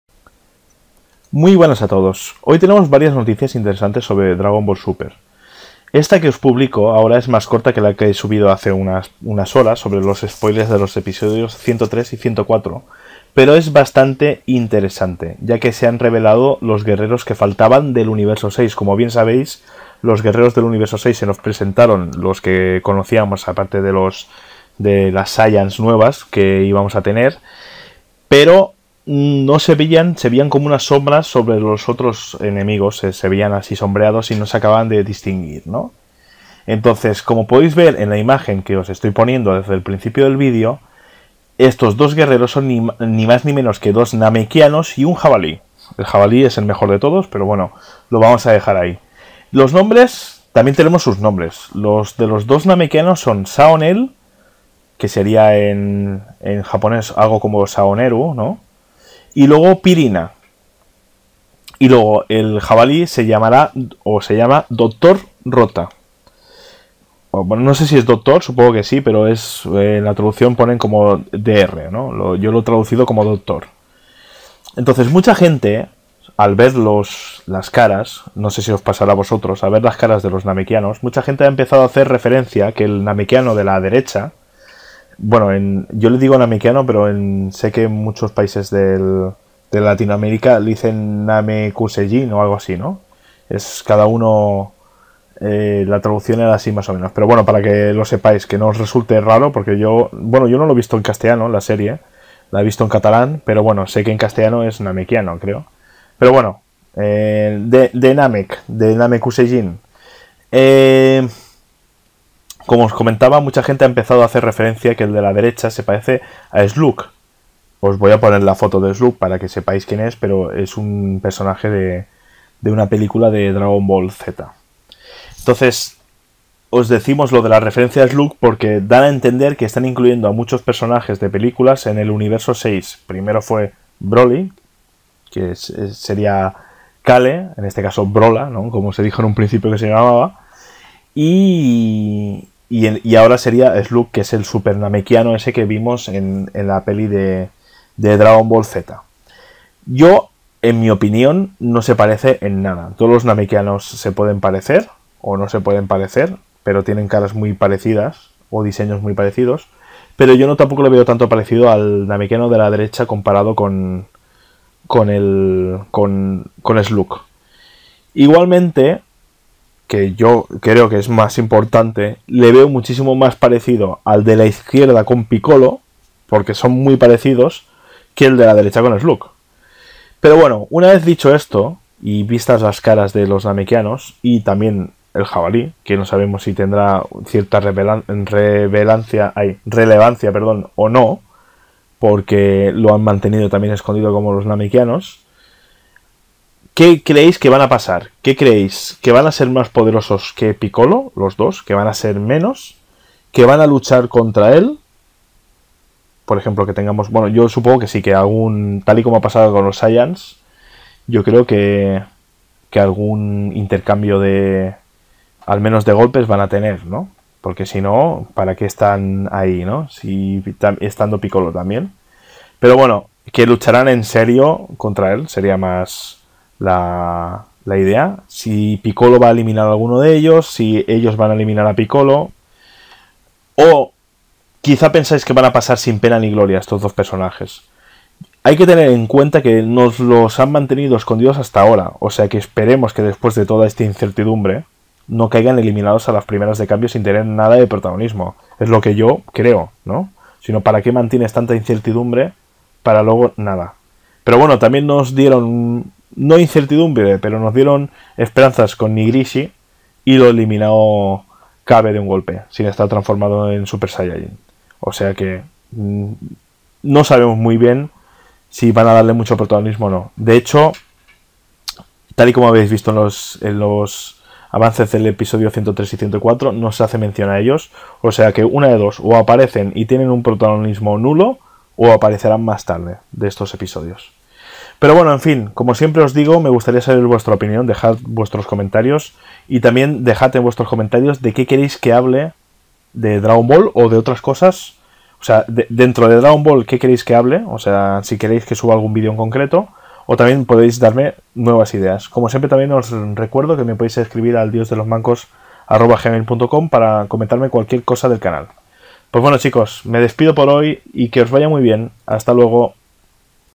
[Dragon Ball Super] Revelada raza y aspecto de los 3 guerreros restantes del Universo 6
Os dejo un vídeo con toda la información que se conoce de los 3 guerreros restantes del universo 6!!